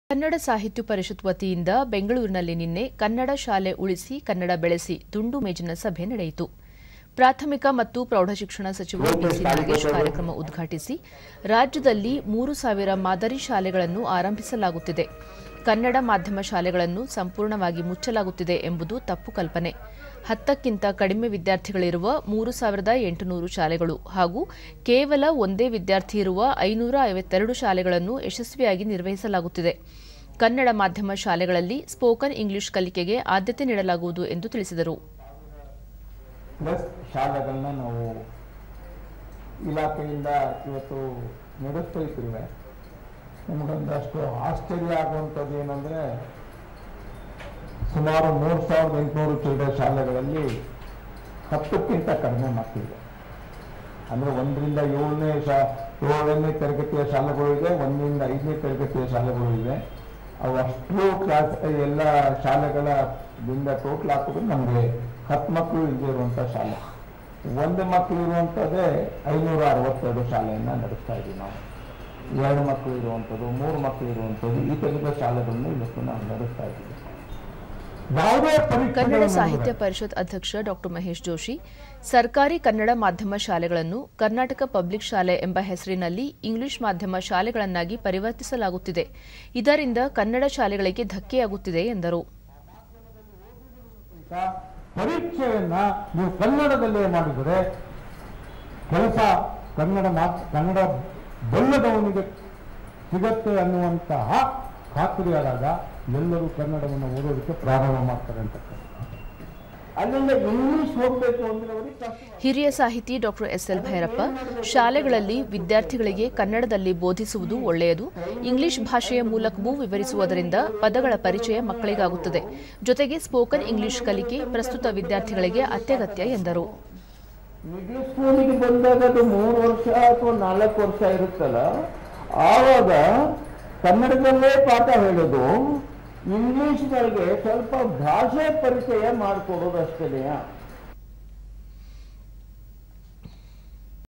ಕನ್ನಡ ಶಾಲೆ ಉಳಿಸಿ ಕನ್ನಡ ಬೆಳೆಸಿ - ದುಂಡು ಮೇಜಿನ ಸಭೆ;ಕಡಿಮೆ ವಿದ್ಯಾರ್ಥಿಗಳಿದ್ದರೂ ಕನ್ನಡ ಶಾಲೆ ಮುಂದುವರಿಸಲು ಬದ್ಧ
ಕನ್ನಡ ಸಾಹಿತ್ಯ ಪರಿಷತ್ತು ವತಿಯಿಂದ ಬೆಂಗಳೂರಿನಲ್ಲಿ ನಿನ್ನೆ ಕನ್ನಡ ಶಾಲೆ ಉಳಿಸಿ ಕನ್ನಡ ಬೆಳೆಸಿ ದುಂಡು ಮೇಜಿನ ಸಭೆ ನಡೆಯಿತು.
ಪ್ರಾಥಮಿಕ ಮತ್ತು ಪ್ರೌಢ ಶಿಕ್ಷಣ ಸಚಿವ ಬಿ.ಸಿ. ನಾಗೇಶ್ ಕಾರ್ಯಕ್ರಮ ಉದ್ಘಾಟಿಸಿ,
ರಾಜ್ಯದಲ್ಲಿ 3 ಸಾವಿರ ಮಾದರಿ ಶಾಲೆಗಳನ್ನು ಆರಂಭಿಸಲಾಗುತ್ತಿದೆ. ಕನ್ನಡ ಮಾಧ್ಯಮ ಶಾಲೆಗಳನ್ನು ಸಂಪೂರ್ಣವಾಗಿ ಮುಚ್ಚಲಾಗುತ್ತಿದೆ ಎಂಬುದು ತಪ್ಪು ಕಲ್ಪನೆ. 10ಕ್ಕಿಂತ ಕಡಿಮೆ ವಿದ್ಯಾರ್ಥಿಗಳಿರುವ 3 ಸಾವಿರದ 800 ಶಾಲೆಗಳು ಹಾಗೂ ಕೇವಲ ಒಂದೇ ವಿದ್ಯಾರ್ಥಿಯಿರುವ 562 ಶಾಲೆಗಳನ್ನೂ ಯಶಸ್ವಿಯಾಗಿ ನಿರ್ವಹಿಸಲಾಗುತ್ತಿದೆ. ಕನ್ನಡ ಮಾಧ್ಯಮ ಶಾಲೆಗಳಲ್ಲಿ ಸ್ಪೋಕನ್ ಇಂಗ್ಲೀಷ್ ಕಲಿಕೆಗೆ ಆದ್ಯತೆ ನೀಡಲಾಗುವುದು ಎಂದು ತಿಳಿಸಿದರು
ಕನ್ನಡ ಸಾಹಿತ್ಯ ಪರಿಷತ್ ಅಧ್ಯಕ್ಷ ಡಾ. ಮಹೇಶ ಜೋಷಿ, ಸರ್ಕಾರಿ ಕನ್ನಡ ಮಾಧ್ಯಮ ಶಾಲೆಗಳನ್ನು ಕರ್ನಾಟಕ ಪಬ್ಲಿಕ್ ಶಾಲೆ ಎಂಬ ಹೆಸರಿನಲ್ಲಿ ಇಂಗ್ಲೀಷ್ ಮಾಧ್ಯಮ ಶಾಲೆಗಳನ್ನಾಗಿ ಪರಿವರ್ತಿಸಲಾಗುತ್ತಿದೆ. ಇದರಿಂದ ಕನ್ನಡ ಶಾಲೆಗಳಿಗೆ ಧಕ್ಕೆಯಾಗುತ್ತಿದೆ ಎಂದರು.
ಹಿರಿಯ ಸಾಹಿತಿ ಡಾ. ಎಸ್.ಎಲ್. ಭೈರಪ್ಪ,  ಶಾಲೆಗಳಲ್ಲಿ ವಿದ್ಯಾರ್ಥಿಗಳಿಗೆ ಕನ್ನಡದಲ್ಲಿ ಬೋಧಿಸುವುದು ಒಳ್ಳೆಯದು. ಇಂಗ್ಲೀಷ್ ಭಾಷೆಯ ಮೂಲಕವೂ ವಿವರಿಸುವುದರಿಂದ ಪದಗಳ ಪರಿಚಯ ಮಕ್ಕಳಿಗಾಗುತ್ತದೆ. ಜೊತೆಗೆ ಸ್ಪೋಕನ್ ಇಂಗ್ಲೀಷ್ ಕಲಿಕೆ ಪ್ರಸ್ತುತ ವಿದ್ಯಾರ್ಥಿಗಳಿಗೆ ಅತ್ಯಗತ್ಯ ಎಂದರು.(HOLD)